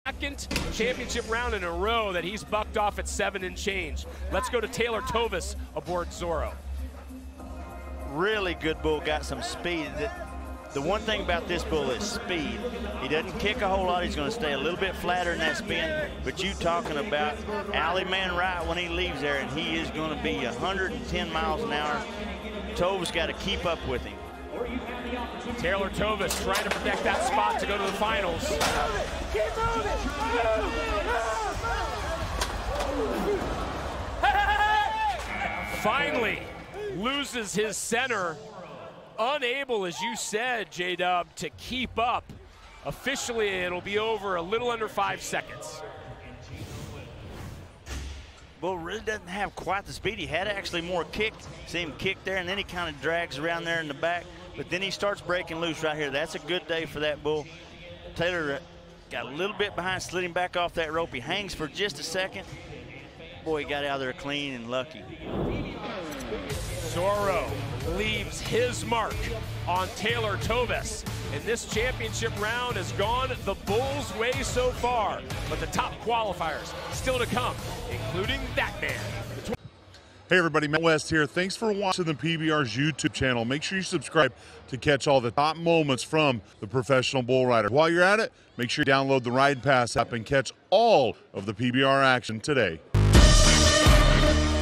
Taylor Toves Gets HUNG UP on Zorro | 2019
Taylor Toves attempted to ride Zorro in Round 2 of the Unleash The Beast Greensboro event. Toves got hung up and didn't make the 8.